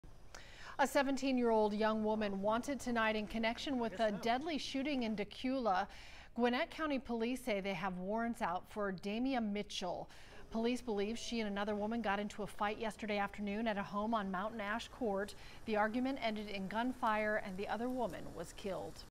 Teen wanted in deadly Dacula shooting
Gwinnett County Police said they have a warrant out for Damia Mitchell.